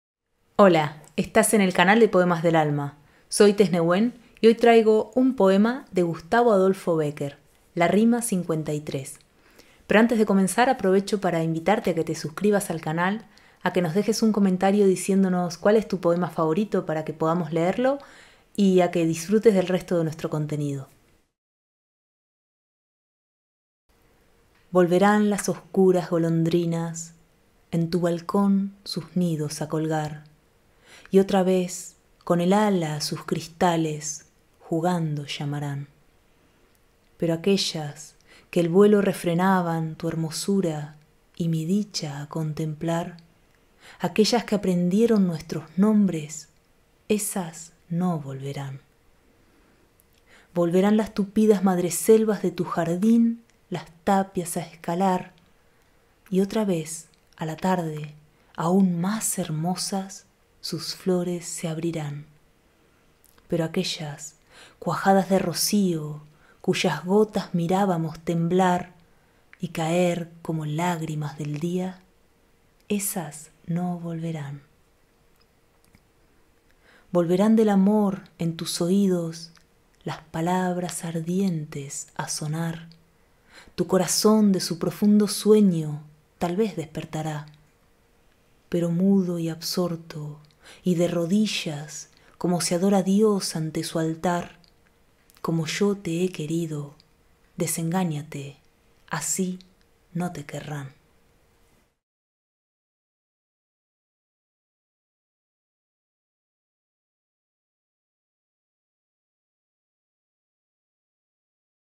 Rima LIII, de Gustavo Adolfo Bécquer
Leemos la "Rima LIII" de Gustavo Adolfo Bécquer, una de sus obras más celebradas.